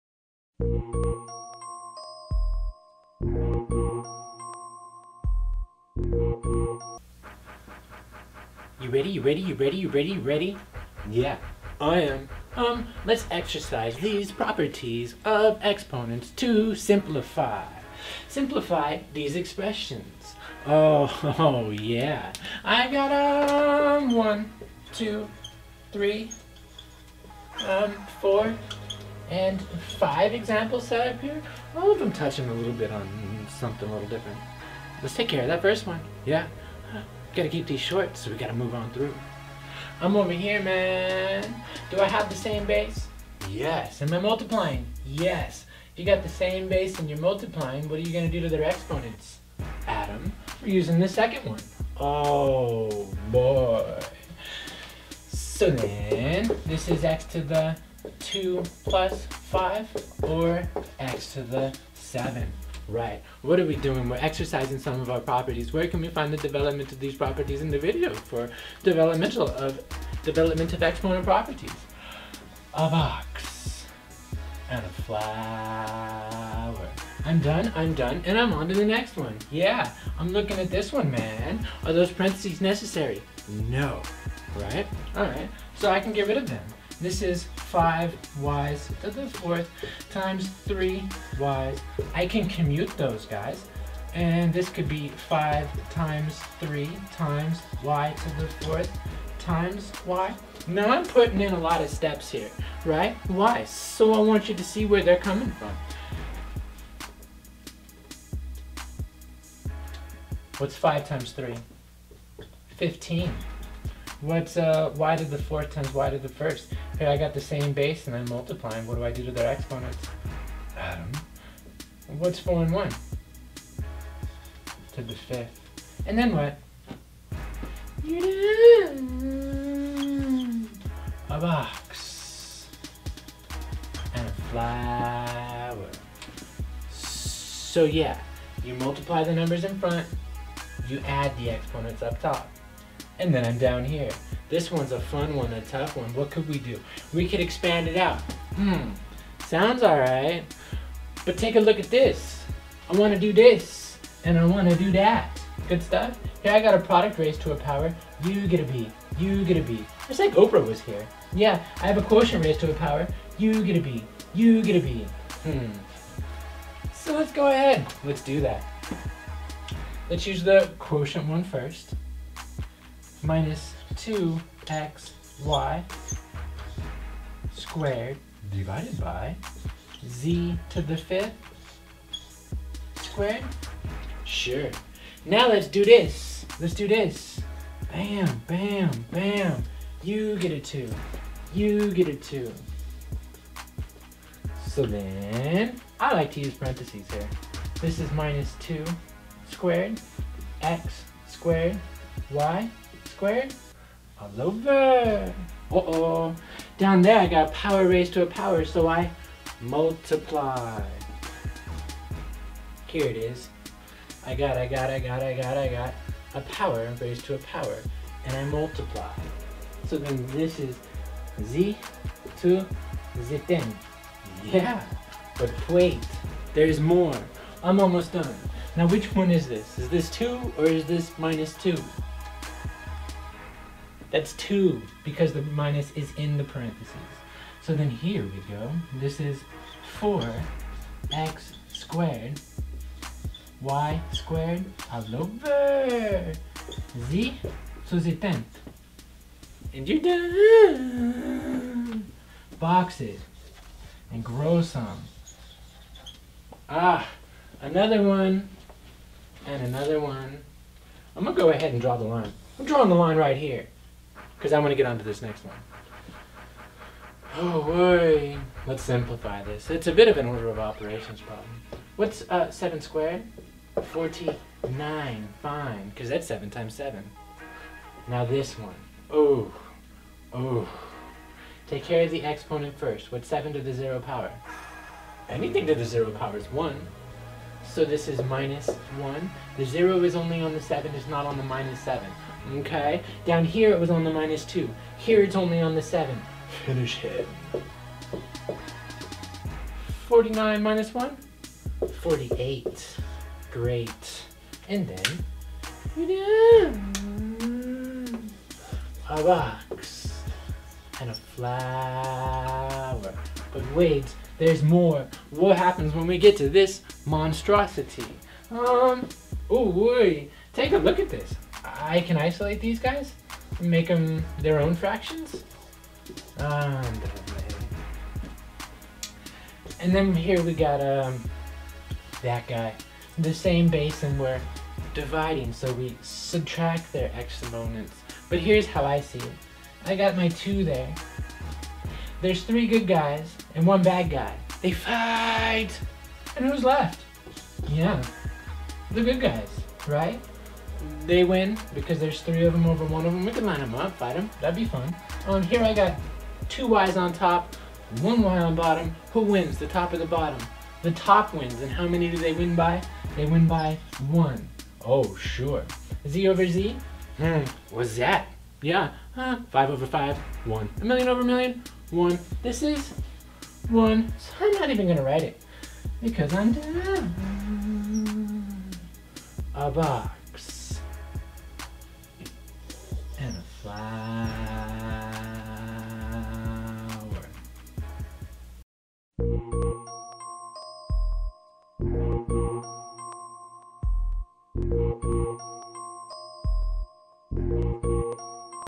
example of exponents 1.mov
This is a collection of examples of exercising the properties of exponents over the integers.